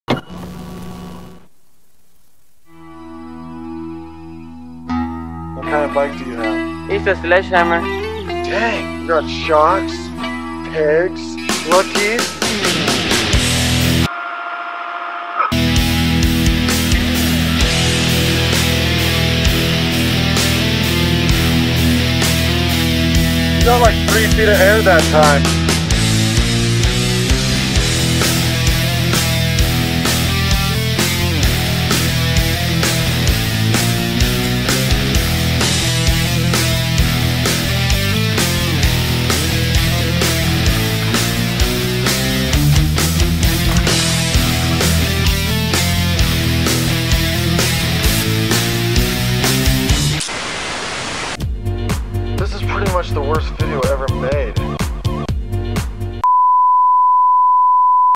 Shot off a rock in '86 // Mountain Bike Short Film
VCR's and VHS tapes. Mountain bikes with pegs. Launching off rocks and sending gap jumps at warp speed. Three feet of air literally! 4:3 and 80's for life.

Gear: Sony a6300 | Sony a6500 | DJI Mavic Pro